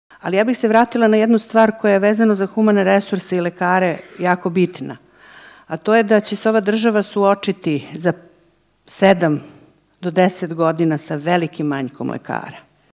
BIZLife konferencija - Budućnost zdravstva i farmaceutske industrije u Srbiji
dr Tatjana Radosavljević, direktor Lekarske komore Srbije - BIZLife konferencija - Budućnost zdravstva i farmaceutske industrije u Srbiji